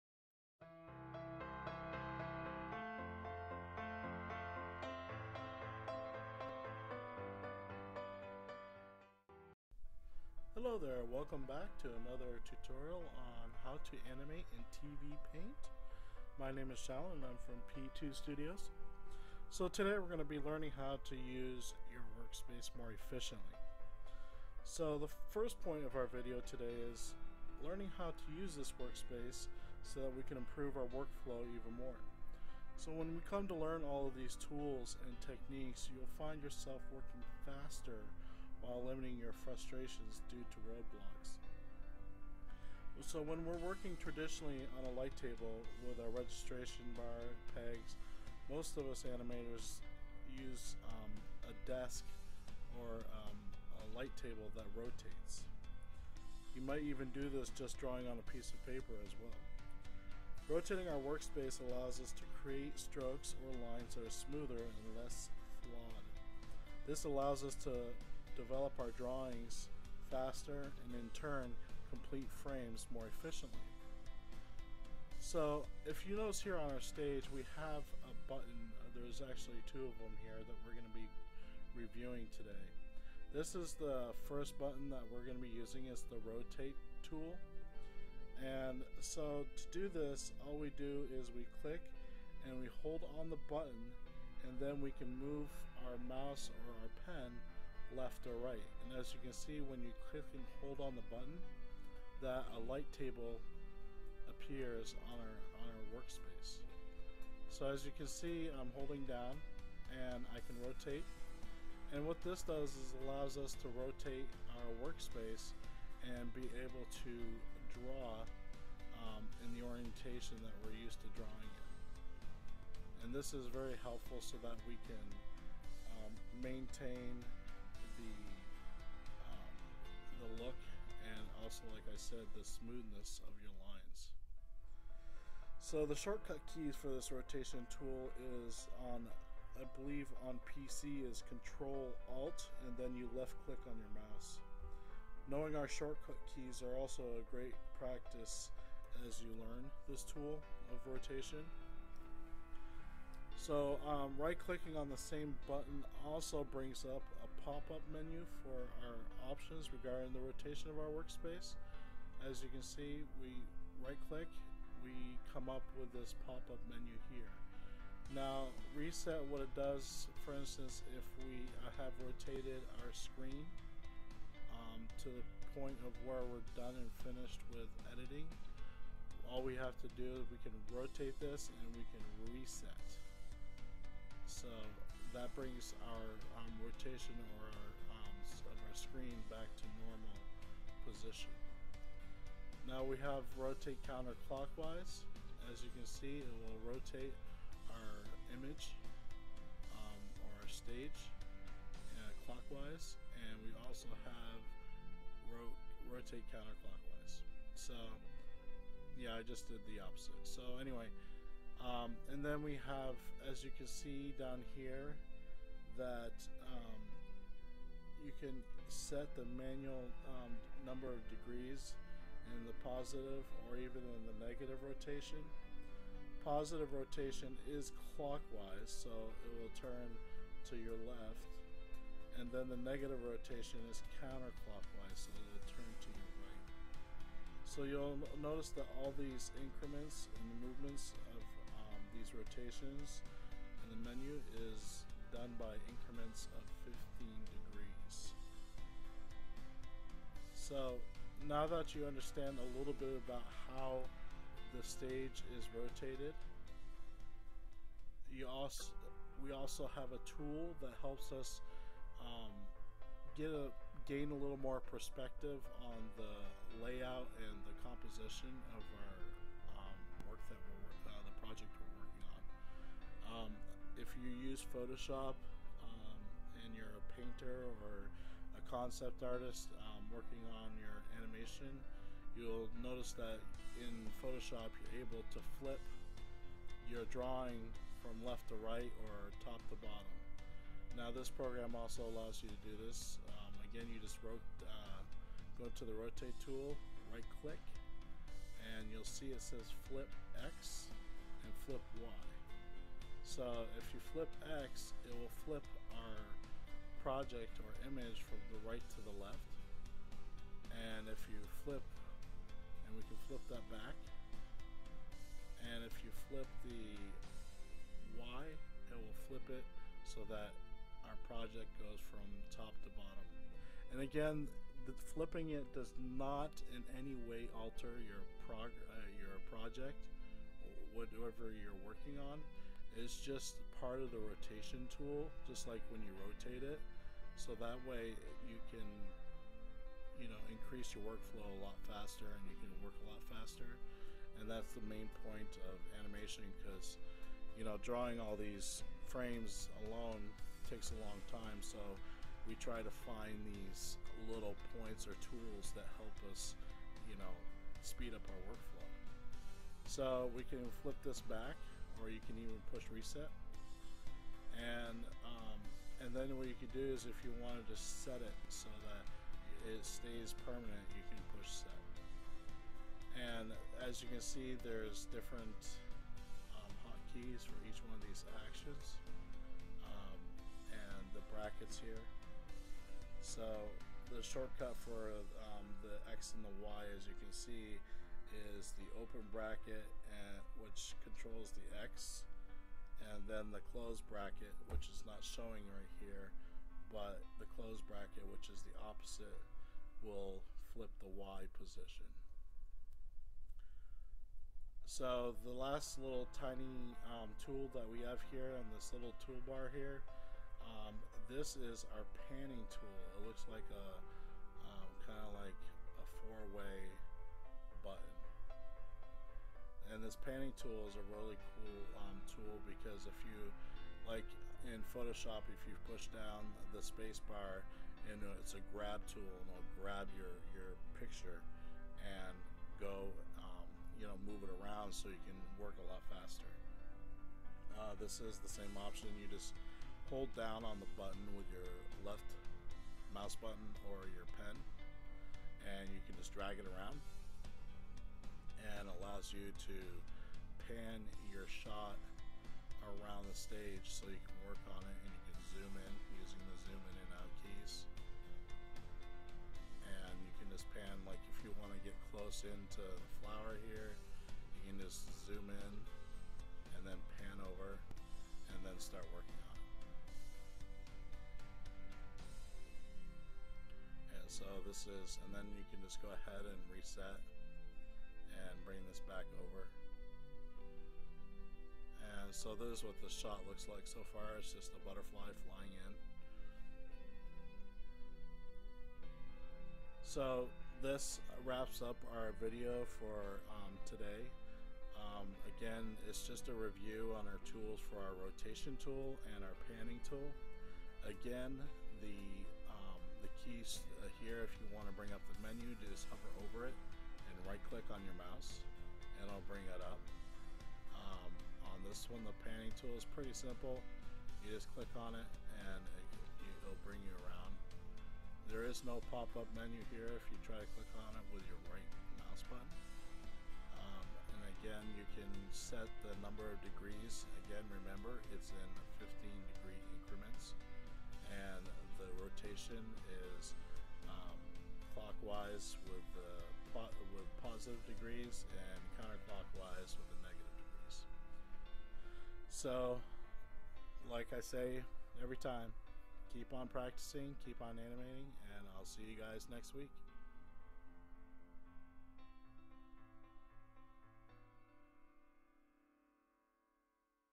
How to use the rotation tool!
In this video learn how to use the rotation tool in your workflow.